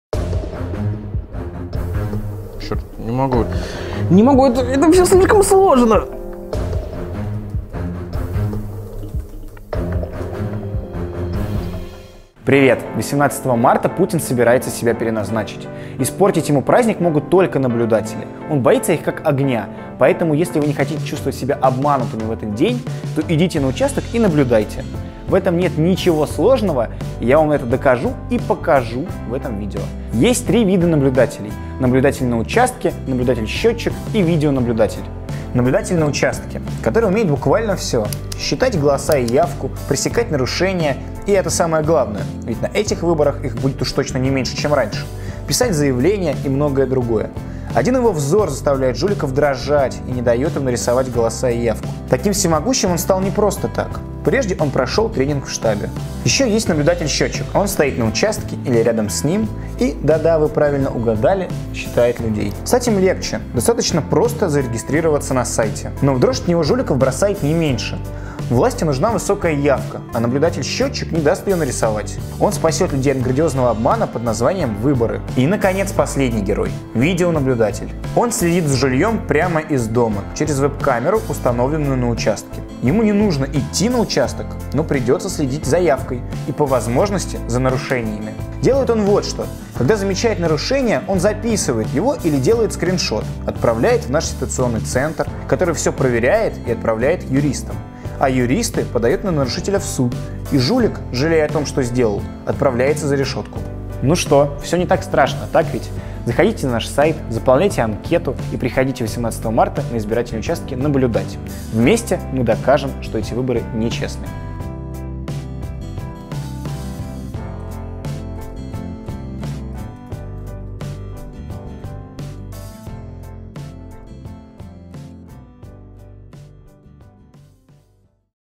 Как заставить Путина страдать?
Многие до сих пор не записались в наблюдатели, потому что считают, что это сложно. На самом деле всё проще, чем вы думаете. И вот почему.